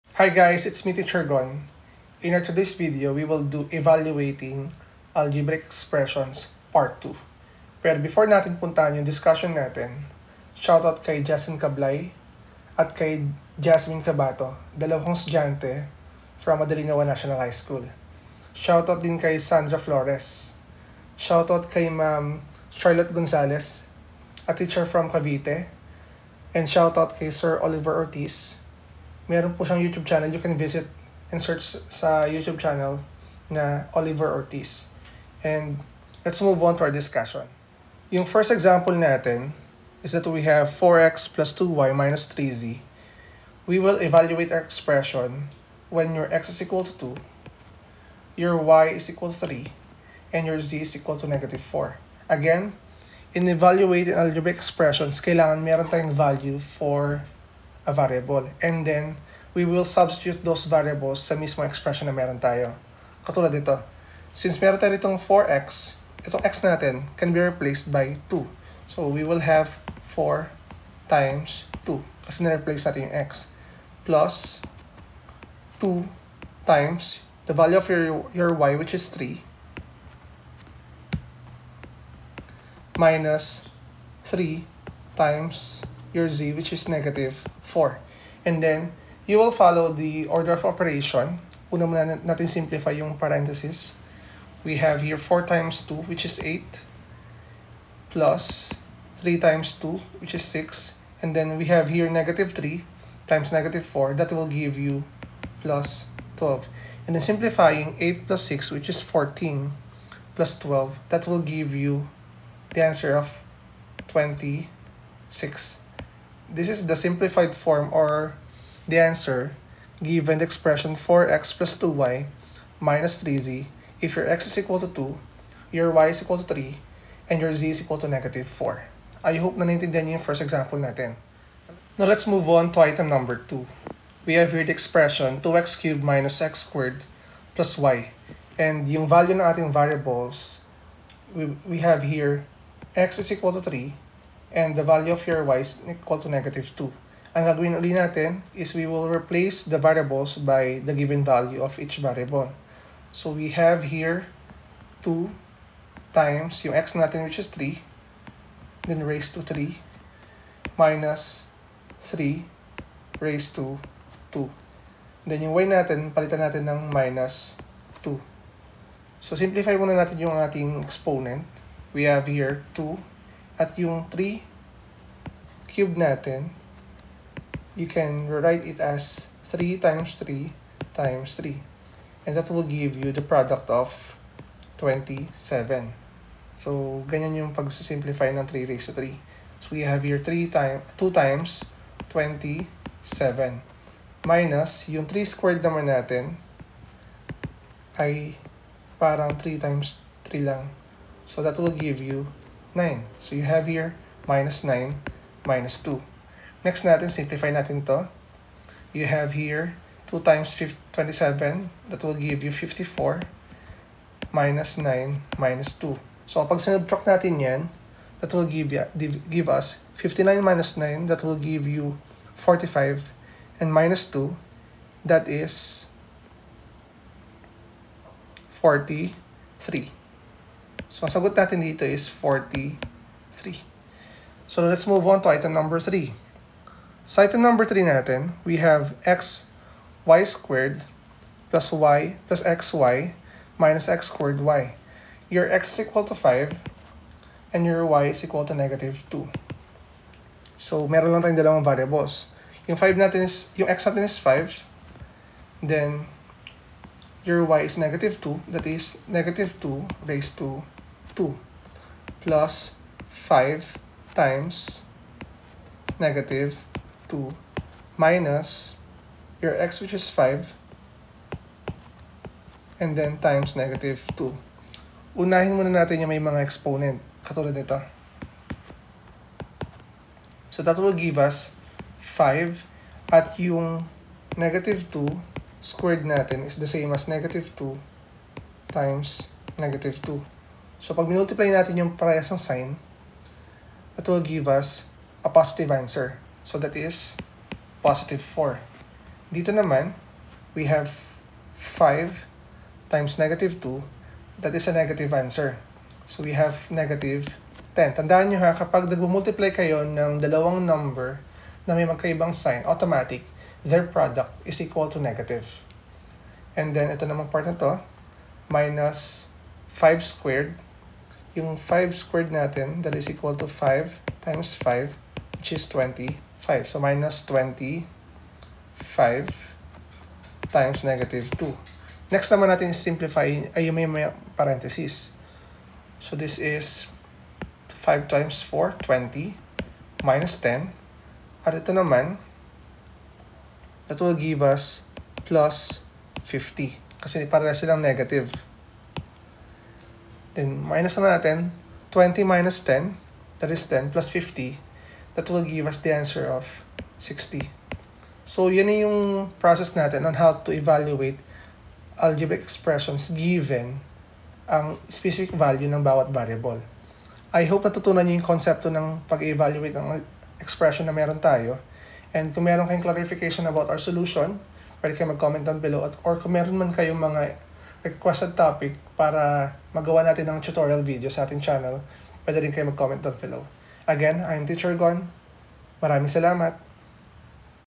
EVALUATING ALGEBRAIC EXPRESSION PART 2 (FILIPINO/TAGALOG)
This video demonstrates how to evaluate algebraic expressions given a variable.

Here is the link of Evaluating Algebraic Expressions Part 1